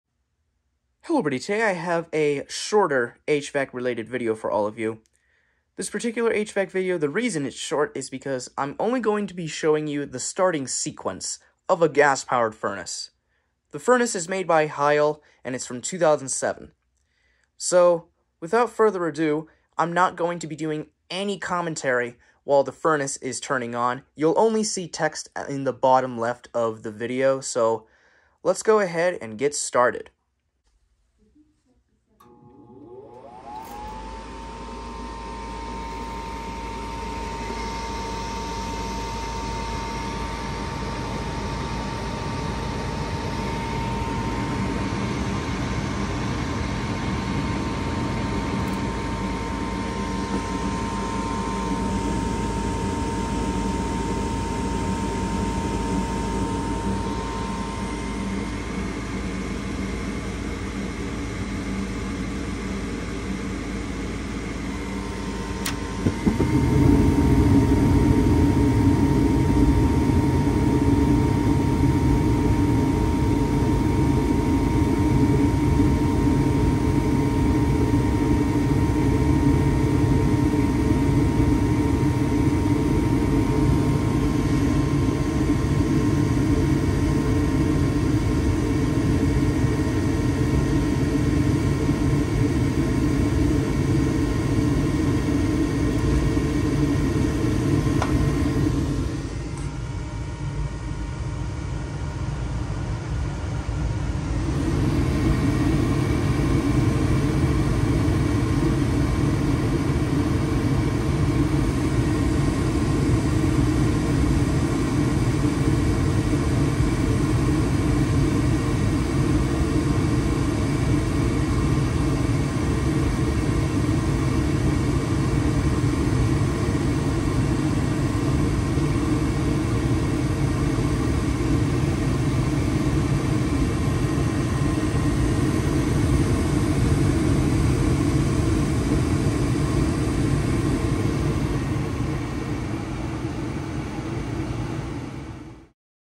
EPIC STARTUP SOUND! All Starting Sequences Of A 2007 Heil Gas Furnace (No Commentary)
In this video w/ no commentary, I will show you the steps of how a gas furnace turns on.